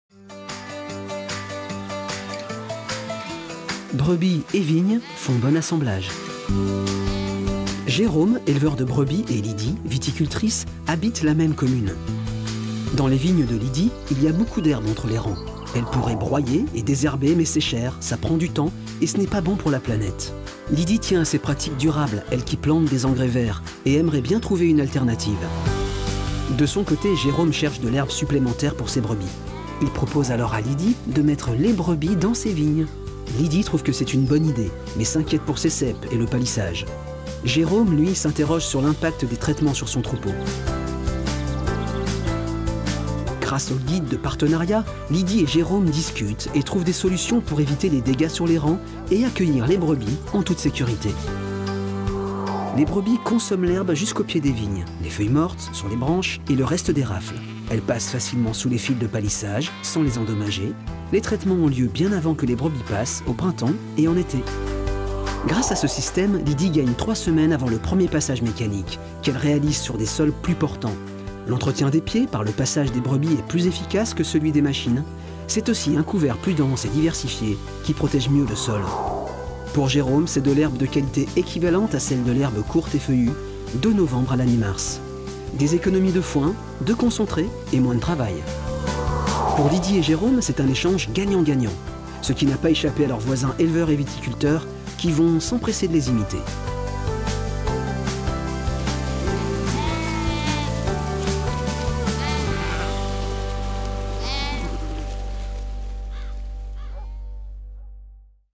Pastoralisme : des conseils pour faire pâturer les vignes par des ovins
Retrouvez les principaux conseils pour mettre en place un partenariat entre éleveurs et viticulteurs à travers ce film d'animation réalisé dans le cadre du projet Brebis_Link porté par la Chambre d'agriculture Dordogne et ses partenaires (Bordeaux Science Agro (33), Chambres d’agriculture de Dordogne, Gironde, Lot, Haute-Vienne, Pyrénées-Atlantiques, Centre interrégional d’information et de recherche en production ovine (CIIRPO), Ecole nationale vétérinaire Toulouse, EPLEFPA de Limoges et du Nord Haute-Vienne, EPLEFPA du Périgord (24) EPLEFPA St-Yrieix la Perche (87) Institut de l’Elevage, Maison de l’Elevage (81), Centre régional d’expérimentation et de démonstration ovine, SICA CREO (24).